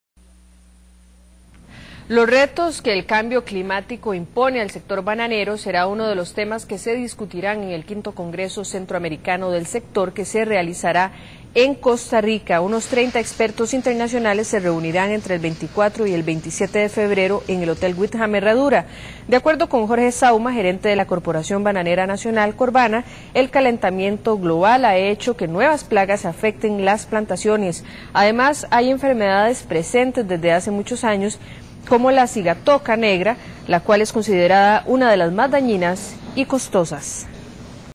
V Congreso Internacional sobre Banano
Los retos que el cambio climático impone al sector bananero, será uno de los temas que se discutirán en el V Congreso Bananero, que se realizará en Costa Rica. Unos 30 expertos internacionales se reunirán entre el 24 y el 27 de febrero, en el hotel Wyndham Herradura.